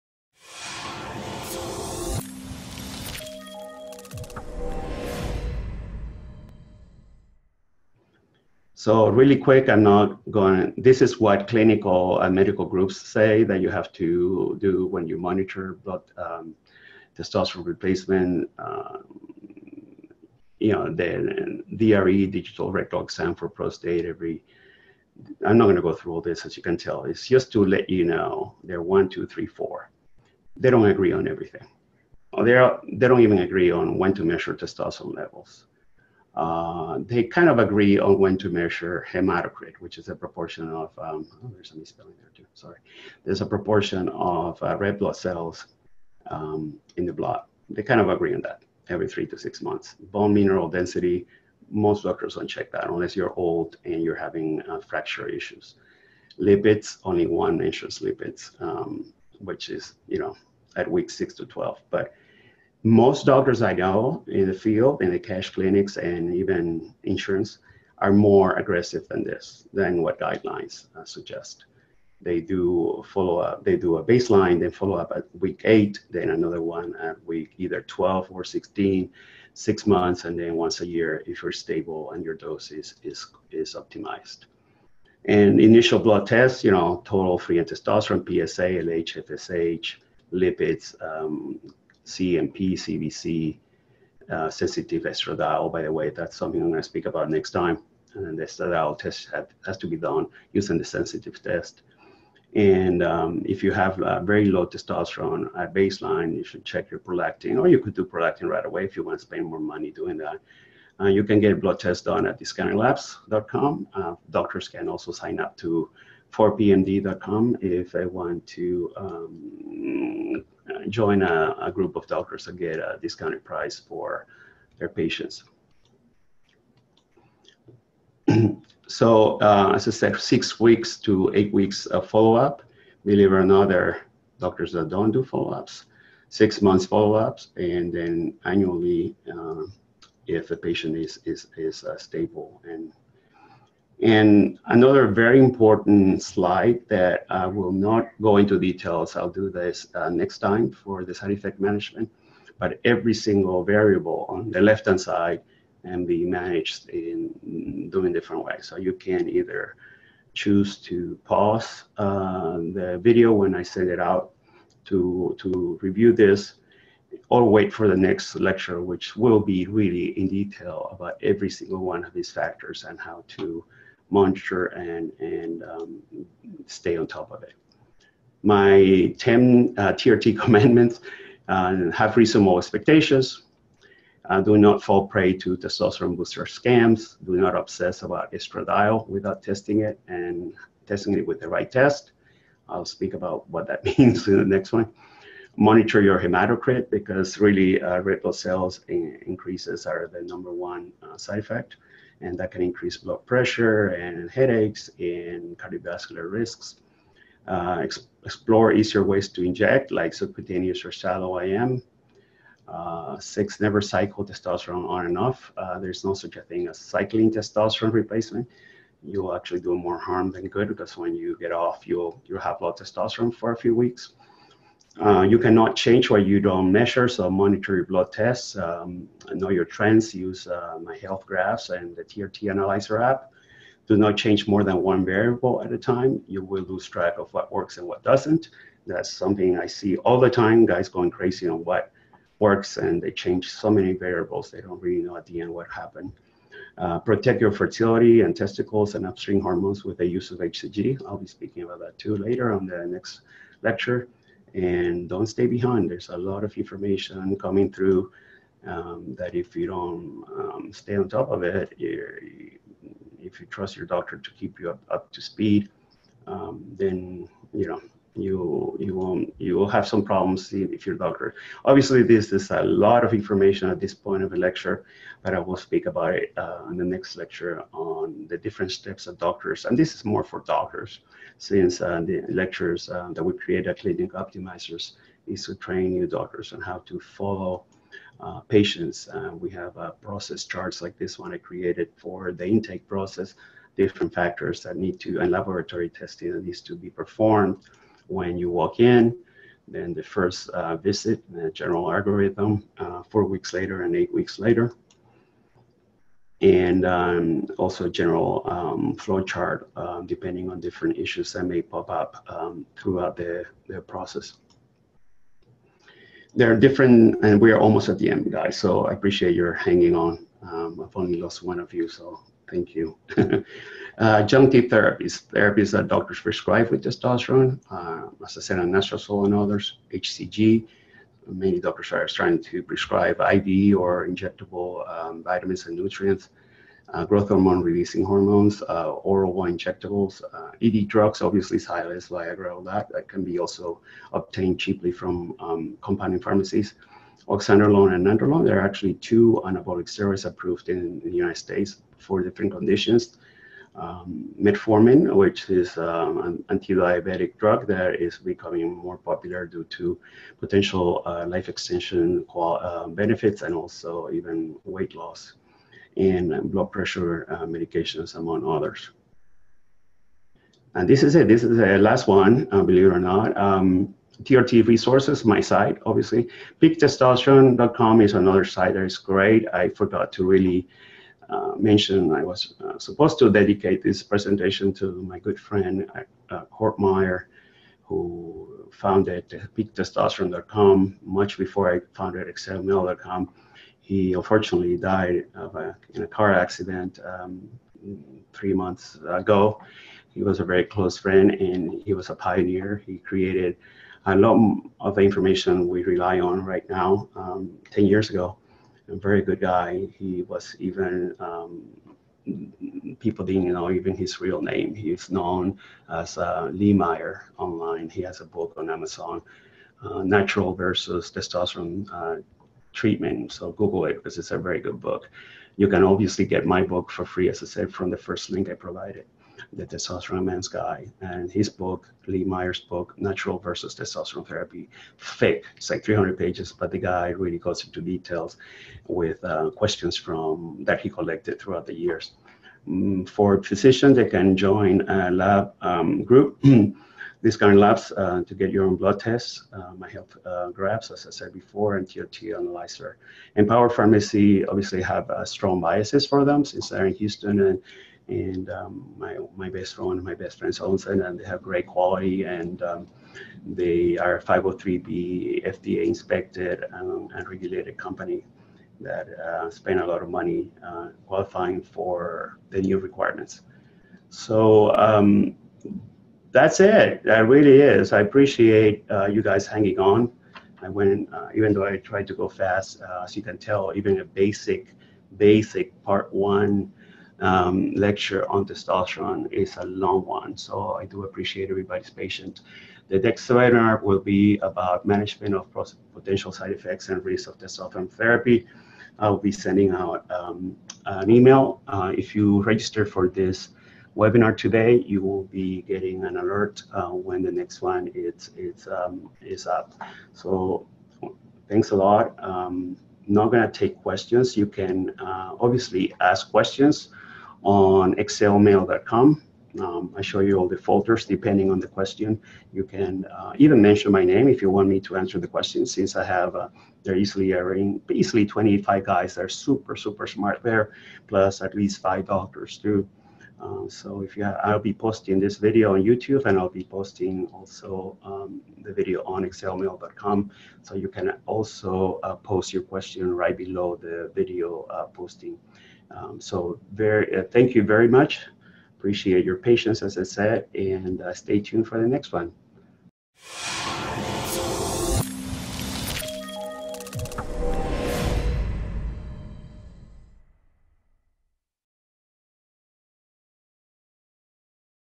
Blood Tests Used to Monitor TRT
Nelson Vergel explains the monitoring blood tests required to maximize benefits and minimize side effects of testosterone therapy.

Summary
This text provides information about blood tests used to monitor testosterone replacement therapy (TRT) and offers recommendations for patients undergoing TRT.

Highlights
💉 Testosterone levels should be measured every three to six months.
💊 Hematocrit levels should be monitored regularly, as an increase in red blood cells can lead to side effects such as high blood pressure and headaches.
🩺 Bone mineral density testing is usually not necessary unless there are fracture issues or the patient is older.
🩸 Lipid testing should be done between weeks six and twelve.
🧪 Initial blood tests should include total testosterone, PSA, hssh lipids, CMP, CBC, and sensitive estradiol.
📈 It is important to track trends and monitor blood tests to understand the effectiveness of the treatment.
🚫 Avoid cycling testosterone and making multiple changes at once, as it becomes challenging to identify what works and what doesn't.

Introduction
Nelson Vergel from ExcelMale.com presents a comprehensive overview of testosterone replacement therapy (TRT). He covers a range of topics from medical monitoring guidelines to side effect management, various therapies, and patient resources.

Medical Monitoring Guidelines
Vergel discusses the varying approaches and consensus among clinical and medical groups concerning TRT monitoring. They don't entirely agree on the optimal times to measure testosterone levels, though there's some consensus on hematocrit (proportion of red blood cells in the blood) measurements every three to six months. Other factors, like bone mineral density and lipids, are less universally assessed. Initial blood tests should include total testosterone, PSA, lipids, CMP, CBC, and sensitive estradiol, with sensitive testing for prolactin in cases of low baseline testosterone.

Patient Monitoring
Doctors typically follow up with patients at weeks 8, 12 or 16, six months, and then annually. This approach allows for adjustment of dosage and monitoring of trends. Vergel emphasizes the importance of not changing more than one variable at a time to accurately track what works.

Side Effect Management
The lecture doesn't delve into detail about managing potential side effects, but it hints at a comprehensive review in an upcoming presentation. Vergel urges patients to monitor their hematocrit as increased red blood cells can lead to elevated blood pressure, headaches, and cardiovascular risks.

10 Commandments of TRT
Vergel shares his tenets of TRT, including having realistic expectations, avoiding scams, monitoring estradiol with the right tests, exploring easier injection methods, protecting fertility with HCG, and staying informed about new information in the field.

Doctors Training
He also covers training doctors to follow patients effectively, discussing the creation of clinical optimizers and process charts for patient intake and follow-up visits.

Common TRT Adjunctive Therapies
Various therapies are highlighted, including anastrozole, HCG, injectable vitamins, nutrients, growth hormone-releasing hormones, ED drugs, anabolic steroids, metformin, and blood pressure medications.

TRT Resources
 He recommends ExcelMale.com and several books on the topic, including his own.

Conclusion
Despite the length and detailed nature of the lecture, Vergel encourages patients and physicians to stay informed and engage with the community on ExcelMale.com for continued learning and support. He anticipates his next lecture will delve into managing potential side effects of TRT.